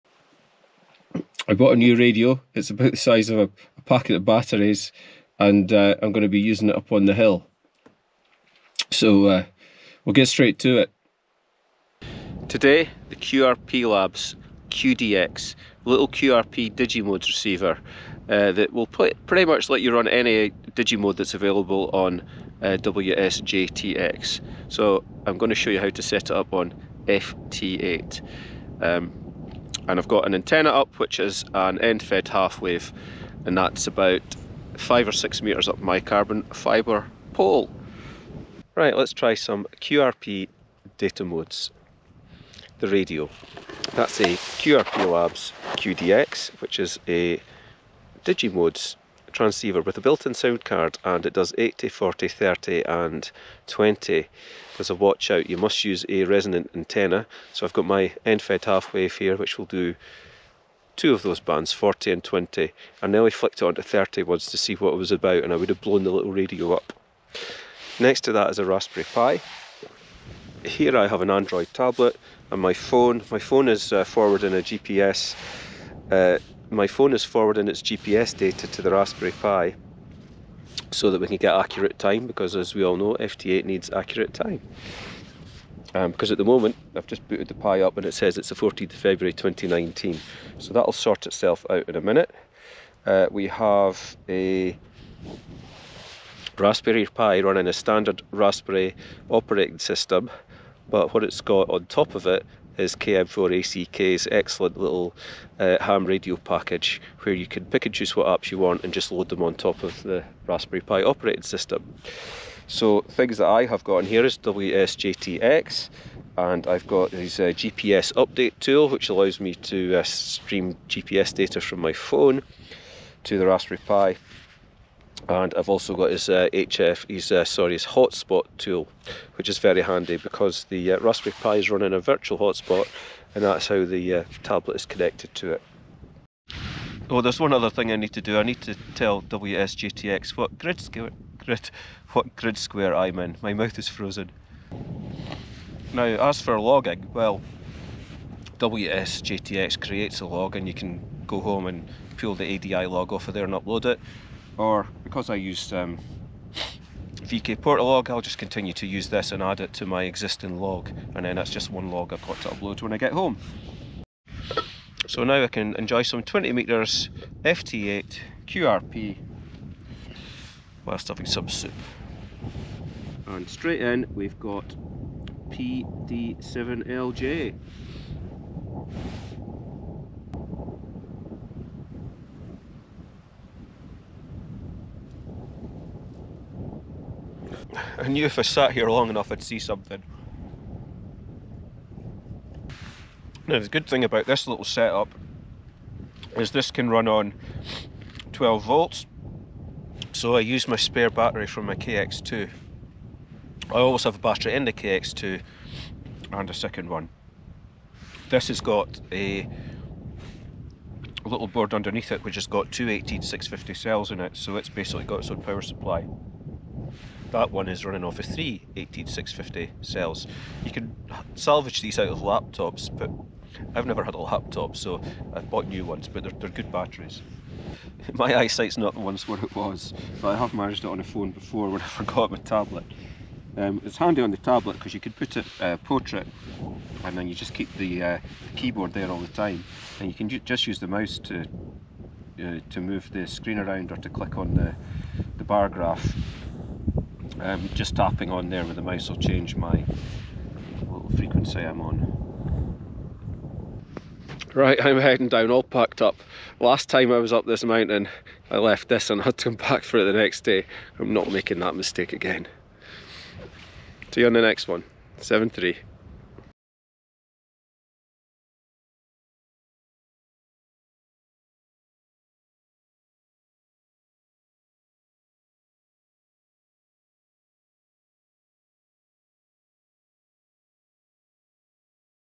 QDX from QRP labs DX from Coyles of Muick
Yje WRP LAbs QDX 5 w digi modes transceiver gets a real world test on a mountain top.